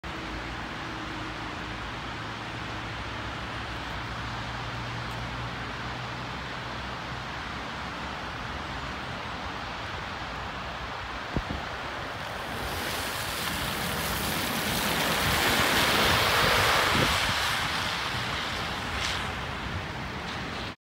Thames Valley Police - roads policing unit - BMW 330D responding
Thames valley police turning out from three mile cross traffic base
Plate : GN19 CZC
Type : Roads Policing Unit
Vehicle : BMW 530D

Follow me @thamesvalleypolicefireamb